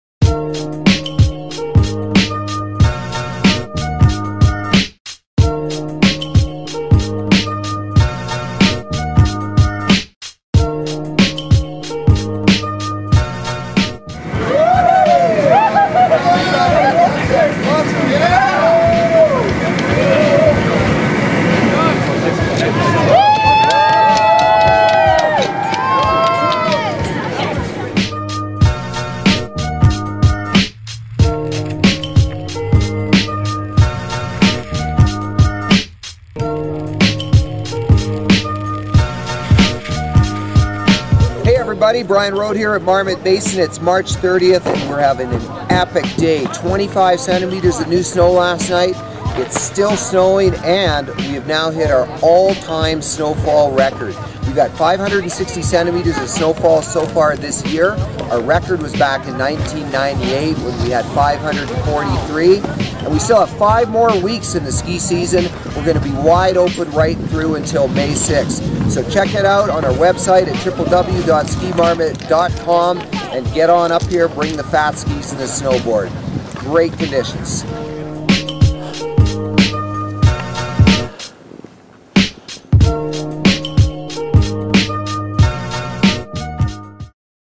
RECORD SNOWFALL AT MARMOT BASIN - MARCH 30, 2012!
Marmot Basin celebrates a 25cm powder day and reaching the all time season snowfall record. Still 5 weeks left in the season for more snow to keep coming!